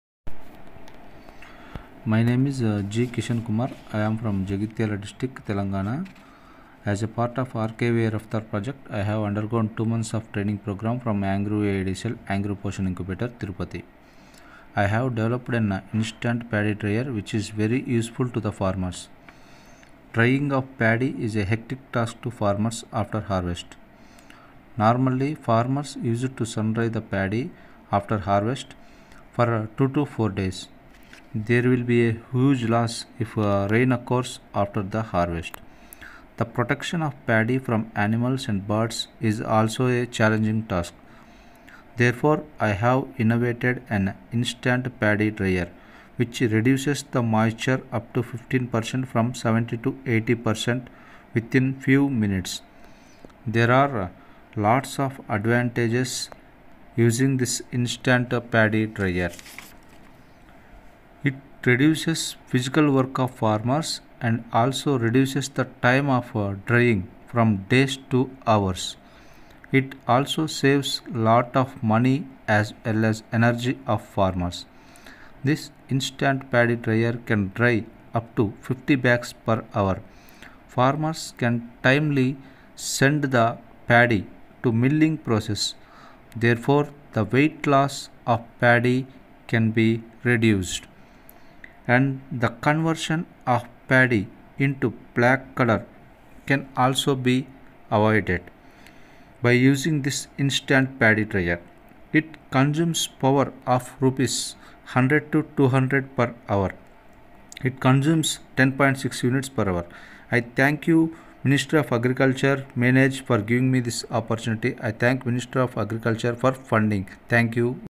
Instant Paddy Dryer- Innovation of Mr. G. Kishan Kumar
Instant Paddy Dryer the paddy grain moisture to less than 17% with in less amount of time and can be operated at field level by the farmers without depending on any other helpers